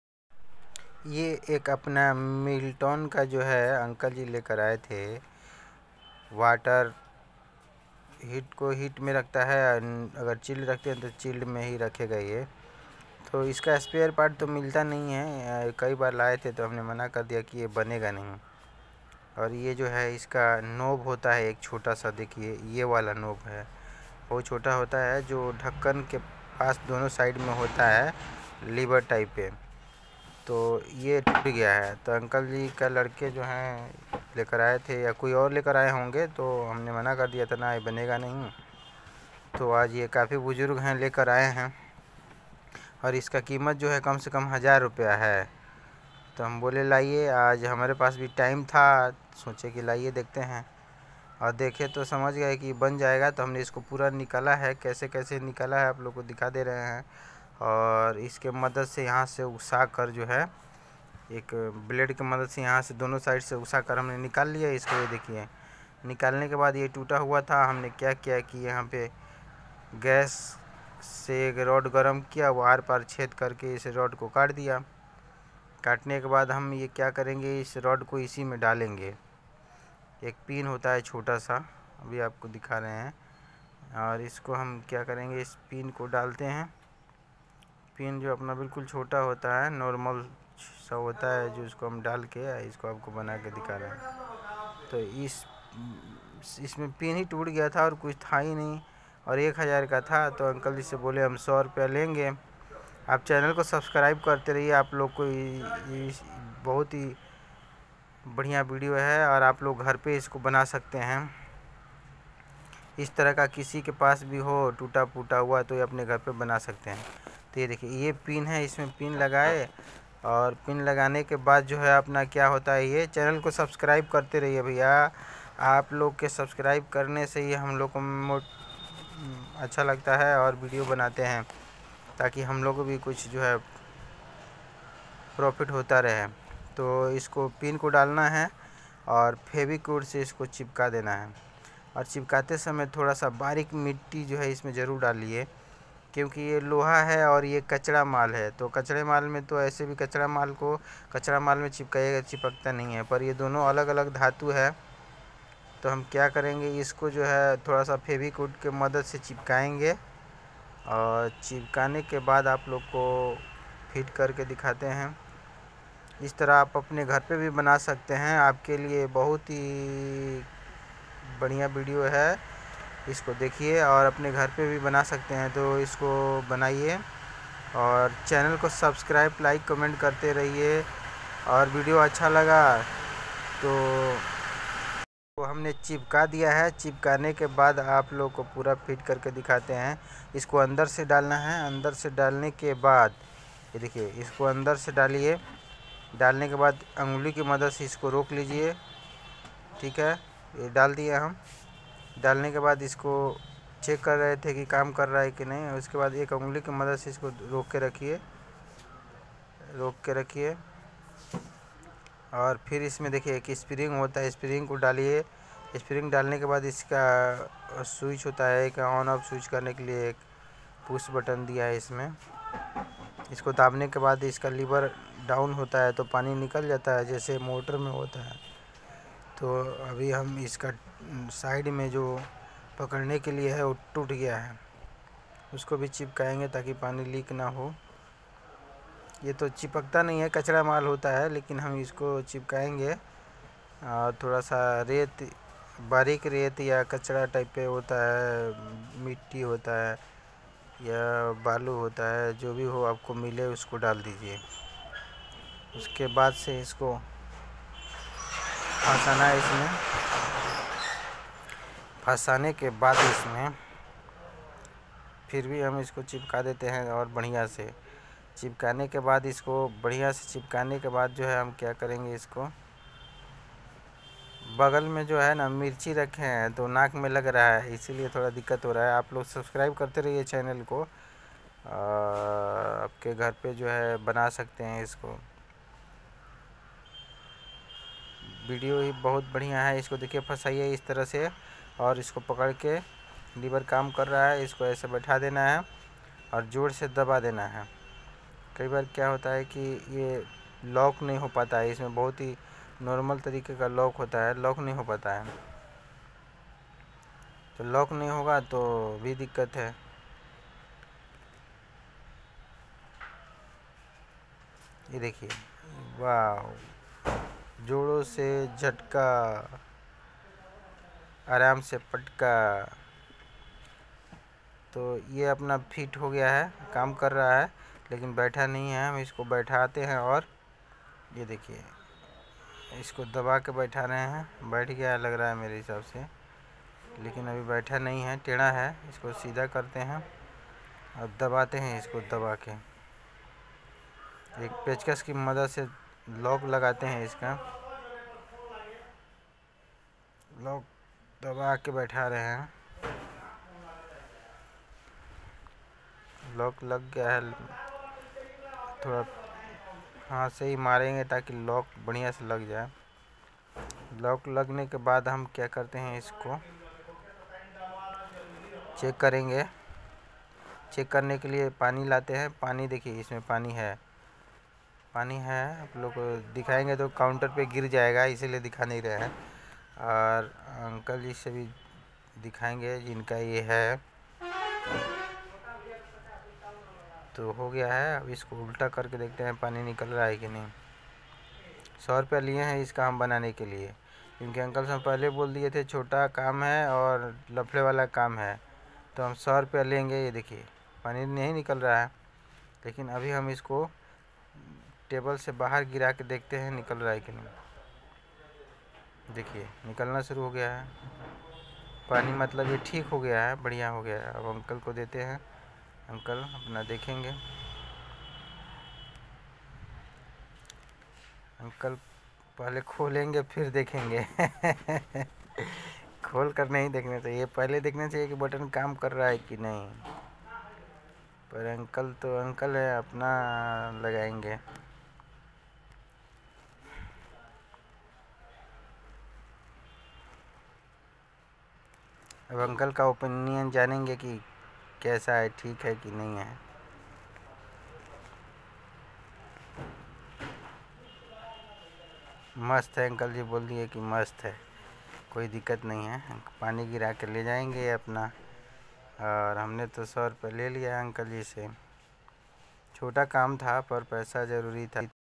Milton water bottle repair
hello guys is video me vacuum flask wali miton ki bottle ko khol ke dikaye hai .